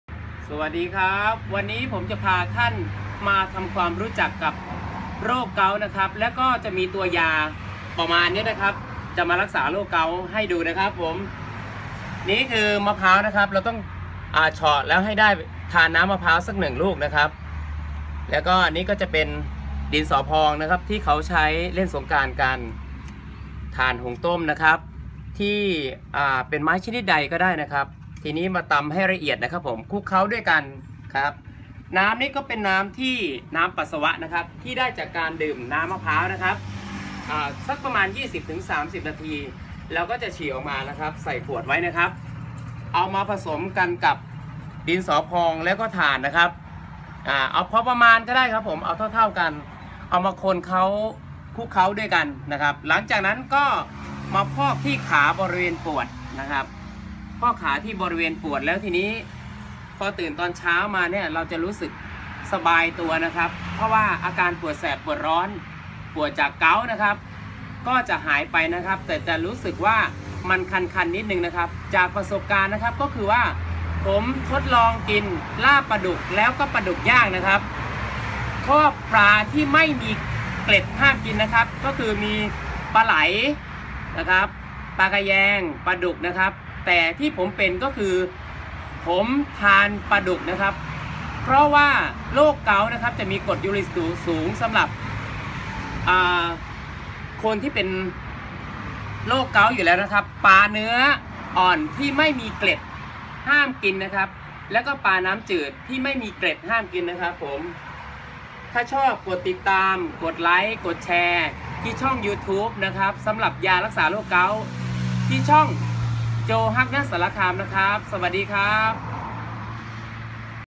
ปวดเก๊าท์ ชั่วข้ามคืนหาย -น้ำปัสสาวะ มะพร้าว ถ่าน ดินสอพอง EP. 01 โรคเก๊าท์ น้ำปัสสาวะบำบัด
โรคเก๊าท์
น้ำมะพร้าว 1 แก้ว
ดินสอพอง
ถ่านหุงต้ม
น้ำปัสสาวะ

ใช้น้ำปัสสาวะบำบัด ด้วยการทานน้ำมะพร้าว 20-30 นาที ฉี่ออกมาเก็บใส่ขวดไว้
ตำดินสอพอง ให้ละเอียด
ตำถ่าน ให้ละเอียด
นำถ่านคลุกเคล้ากับดินสอพอง แล้วเอาน้ำปัสสาวะ ที่ได้จากการดื่มน้ำมะพร้าว เทลงคลุกเป็นต้ม
หลังจากนั้นมาพอกที่บริเวณปวด

สิ่งต้องห้ามรับประทาน
ปลาที่ไม่มีเกล็ดห้ามทาน เช่น ปลาดุก ปลาไหล ปลาแขยง